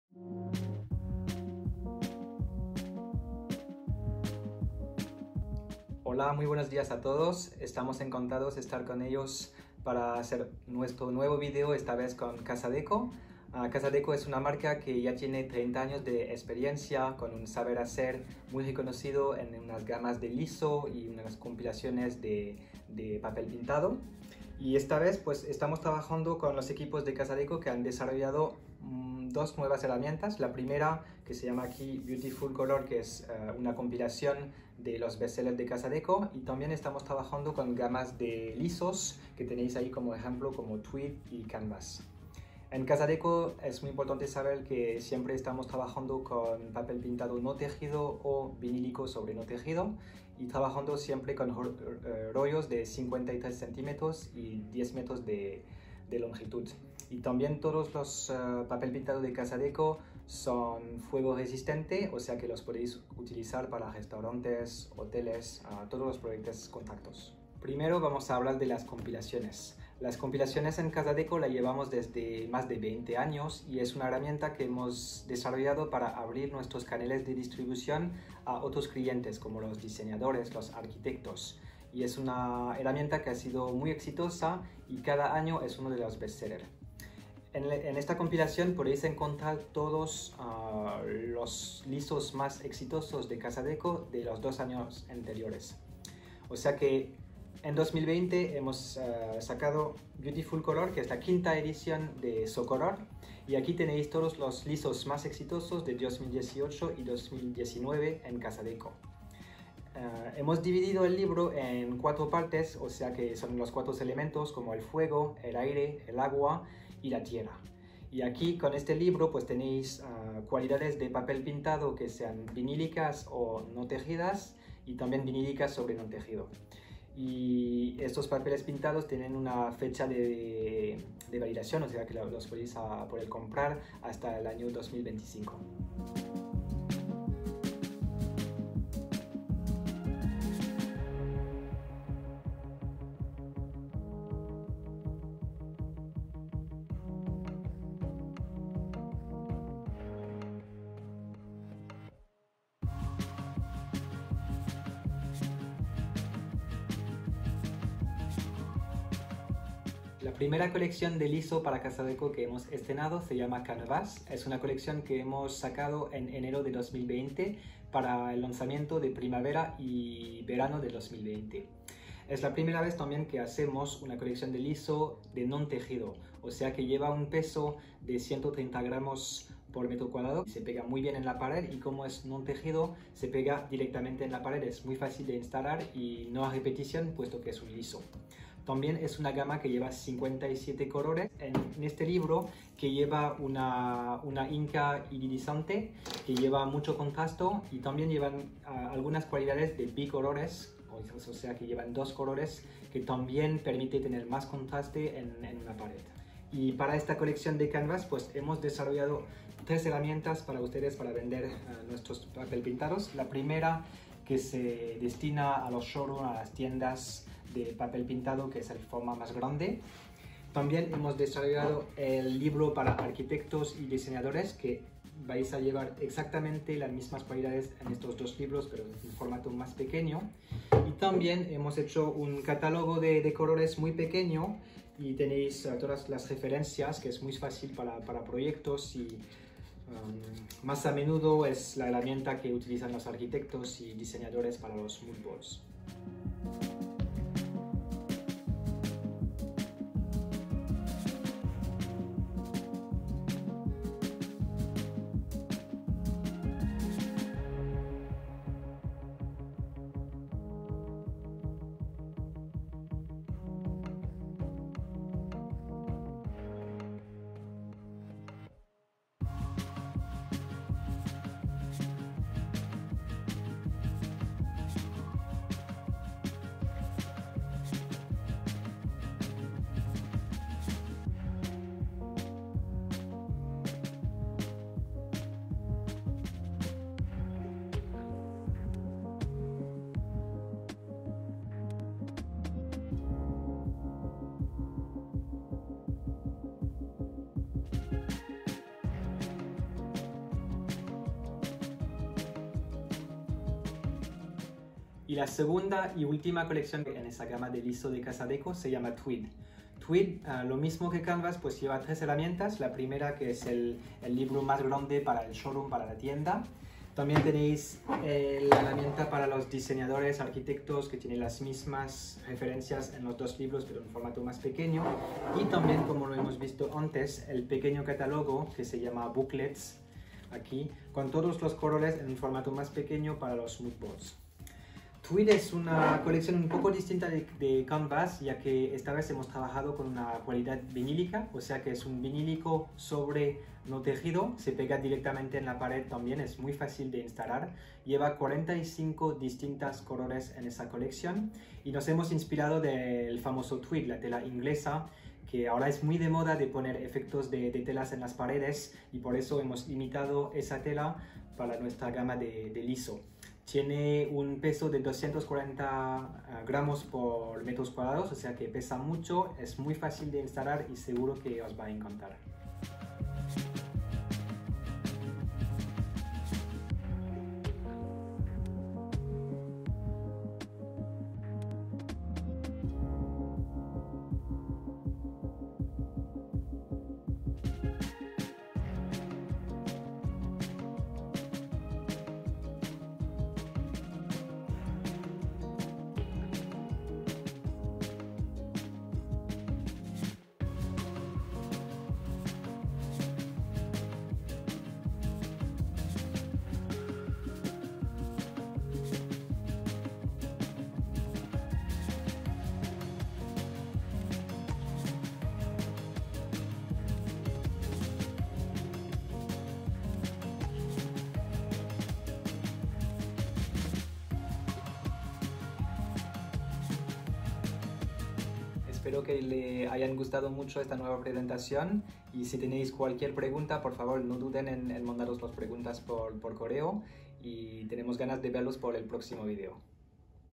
CASADECO - Focus - Les Unis / Los lisos - Papel Pintado - Espanol
Video de presentacion de la gama de papel pintado lisos de CASADECO.